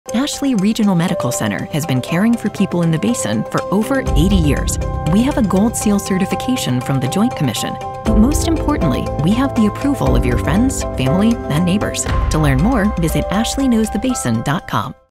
We Know The Basin - Ashley Regional Medical Center
Ashley Regional Medical Center has been serving people in The Basin for over 80 years. We’ve delivered your babies. We’ve put our entire heart into caring for yours. We’ve mended bones and healed wounds. We couldn’t be prouder of who we are as a community.
We know care. We know you. We know The Basin.